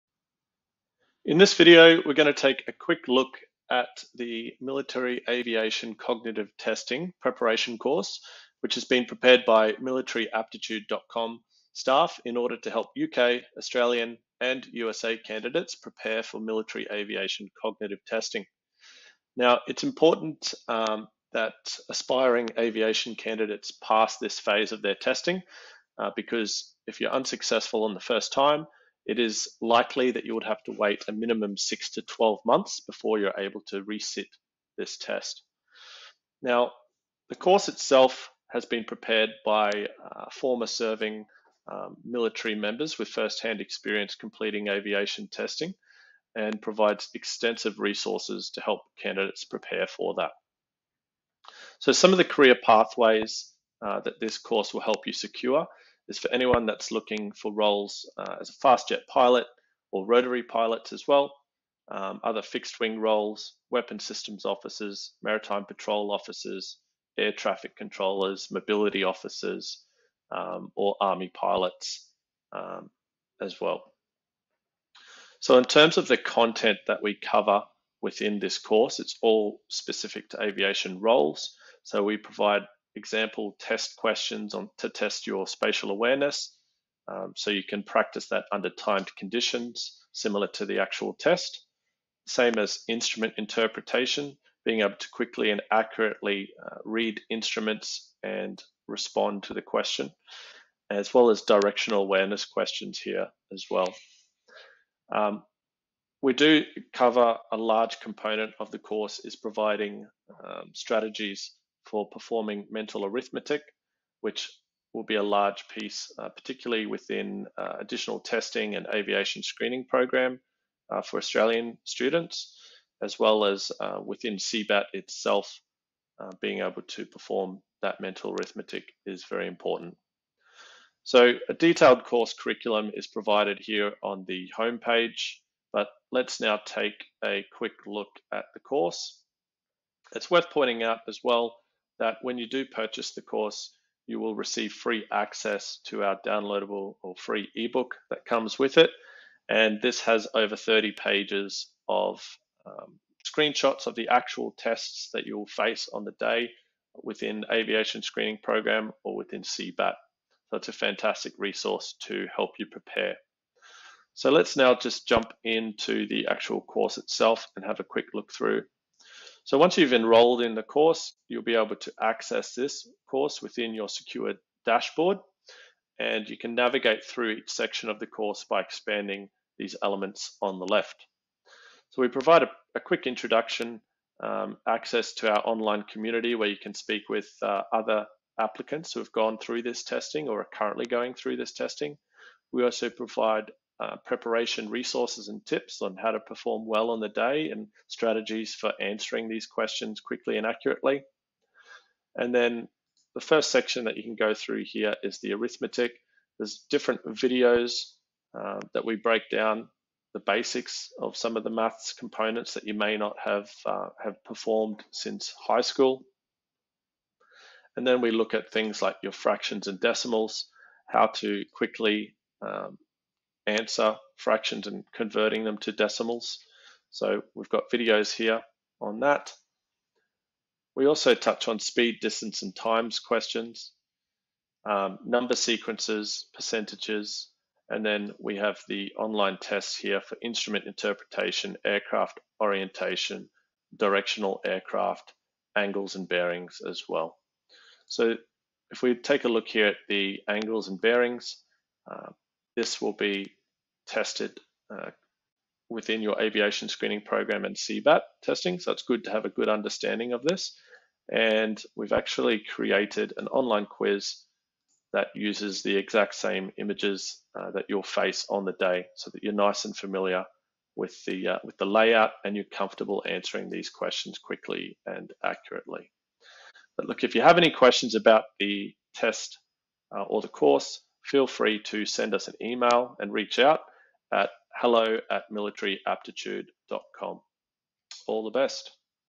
Military Aviation Cognitive Testing (MACTS)
In this video, we look at the online military aviation cognitive testing (MACTS) preparation course provided by military apttiude.com